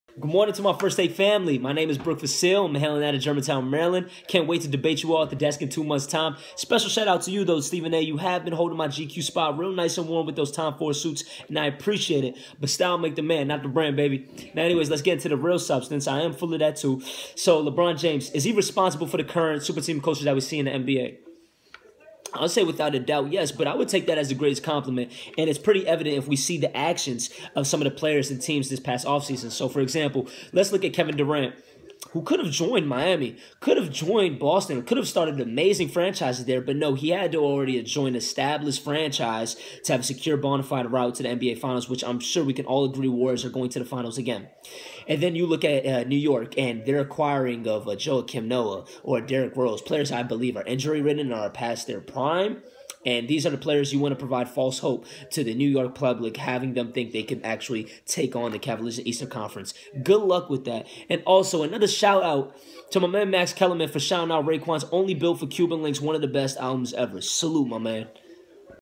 FirstTake/MyTake - Super Team Culture
Is Lebron James responsible for the current super team culture in the NBA?

Expect more debates from myself this week.